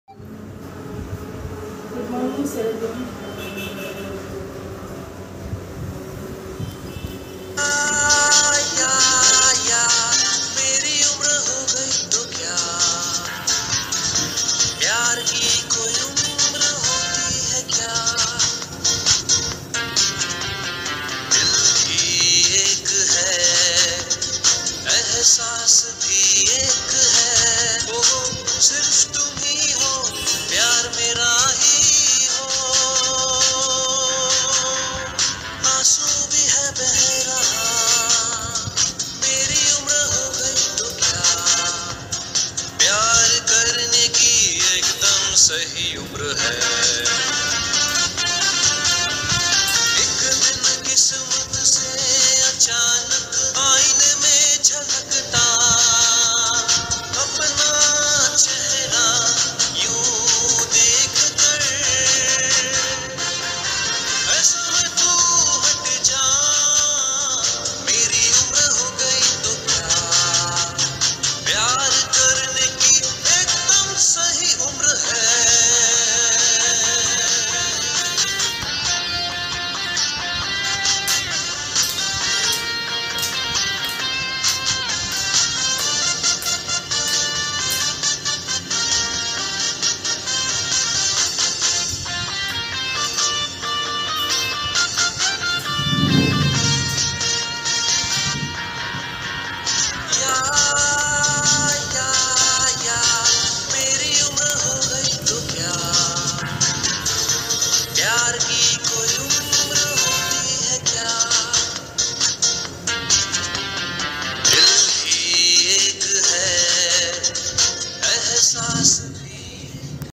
Nilima biswas | Barrackpur | Ceragem Healthy Exercise Challenge | Dil Se Dil Tak
Nilima biswas from Barrackpur had participated in the Ceragem ongoing Event named Ceragem Healthy exercise Challenge. Nilima biswas has done an outstanding job and we are very fortunate that you have participated in the Event!